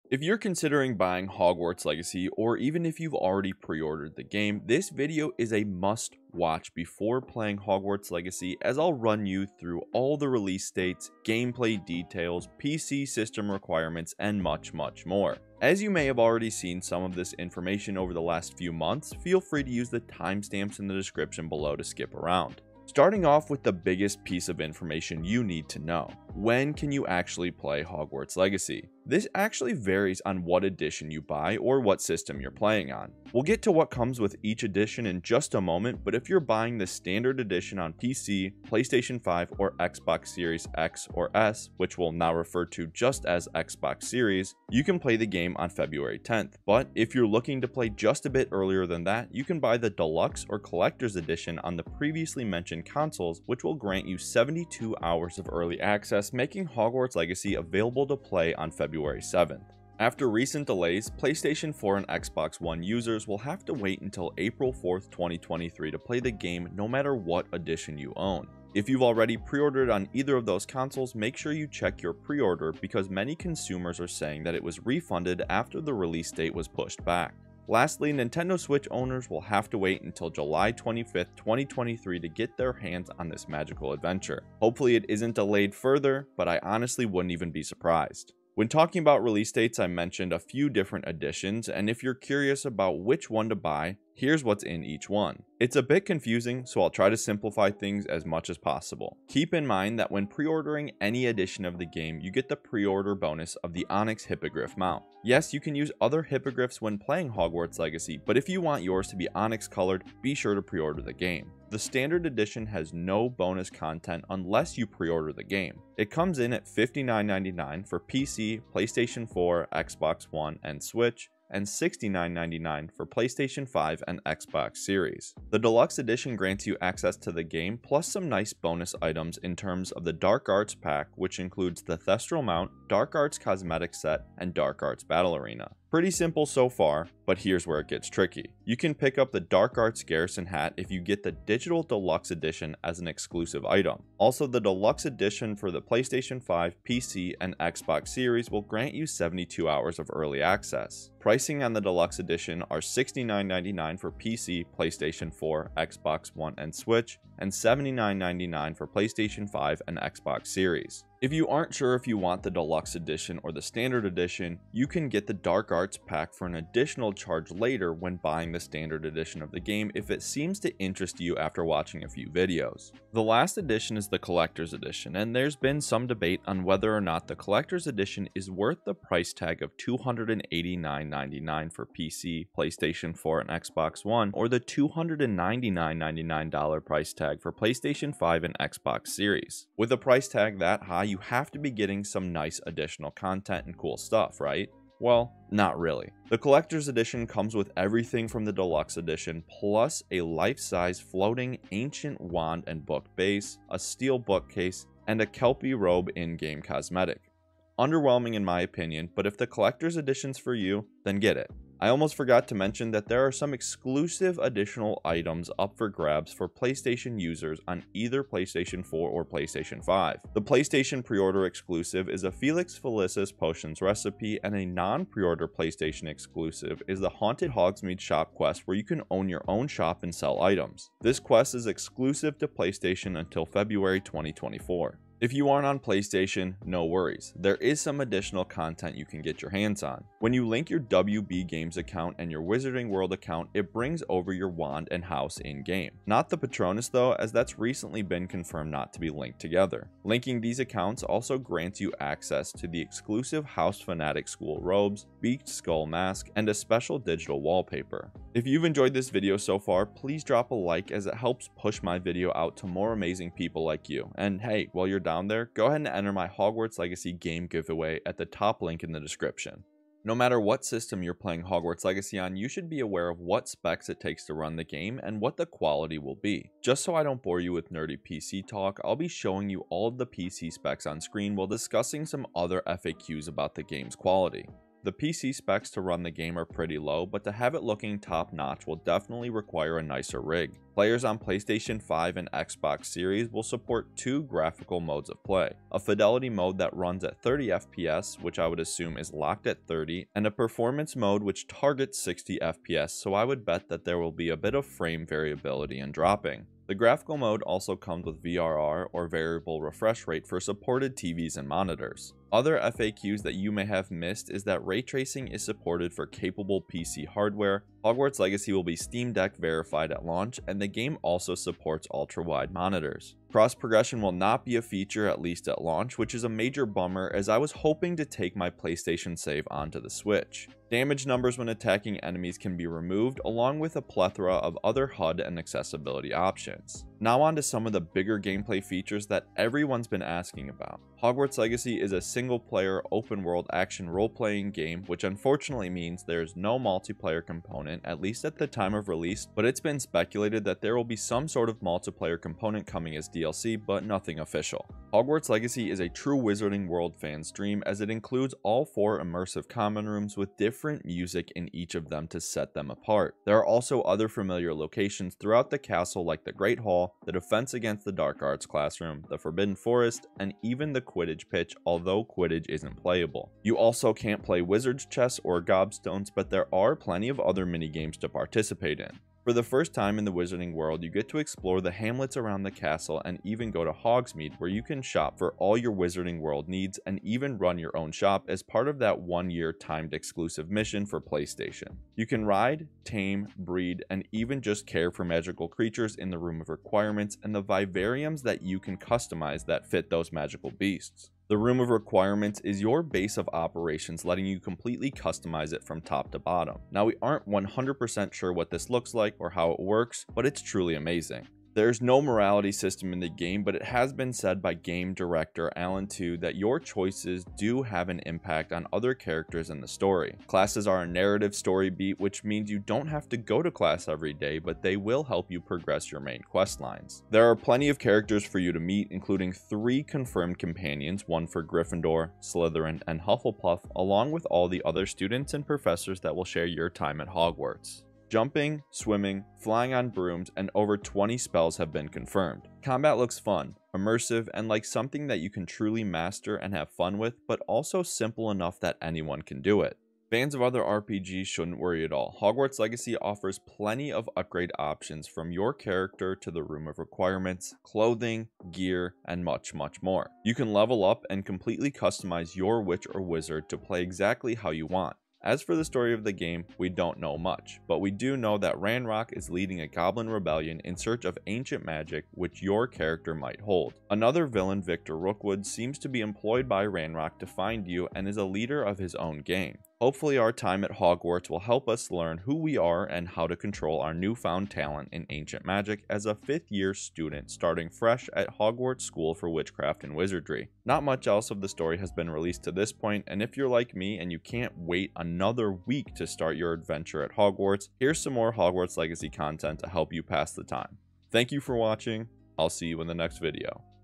Watch This BEFORE Playing Hogwarts Legacy…
Hogwarts Legacy is just a few weeks away at this point and if you are on the fence about buying the game… here is a complete game preview of everything you need to know before playing Hogwarts Legacy!

Are you new around here? I make nerdy content of all kinds such as news, reviews, entertainment, gameplay and more!
Timestamps:

0:00 Intro
0:22 Release Date
1:28 Each Edition
3:32 Special Content
4:30 Specs and Graphics
5:12 FAQs
7:45 Story